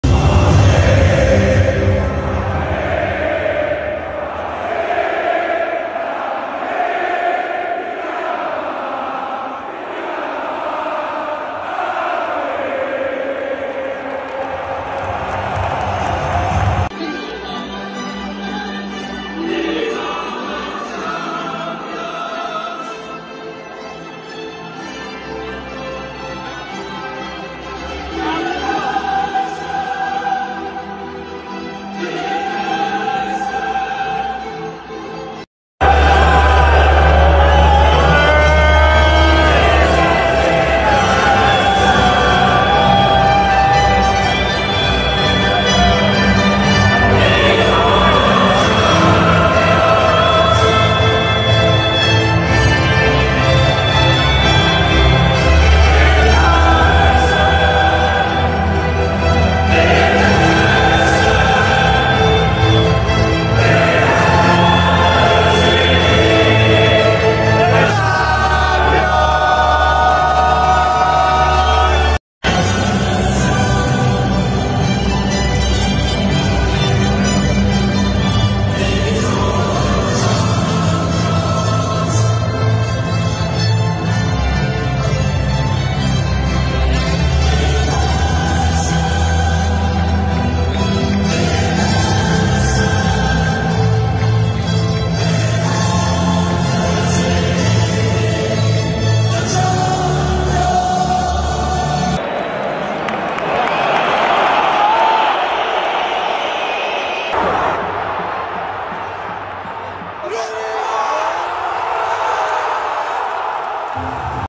Recibimiento Real Madrid vs Manchester city / tifo mosaico Real Madrid
El espectacular tifo en el Santiago Bernabéu se ha desplegado en el fondo sur del estadio, donde miles de aficionados han levantado unas cartulinas, formando el escudo gigante del Real Madrid y que ha ocupado prácticamente todo el fondo de la granda a lo ancho y a lo alto.